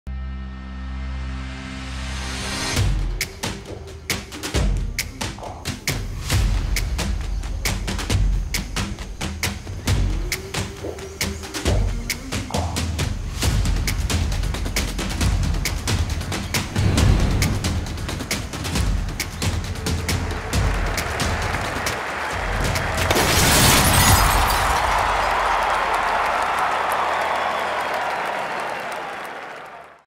IQbuds² MAX and IQstream TV: Baseball
Without being able to attend baseball games in person, enjoy the next best thing with IQstream TV and IQbuds² MAX wireless earbuds. From the crack of the bat to the snap of the catcher's mitt, bring the sounds of the ballpark to your ears in high-definition.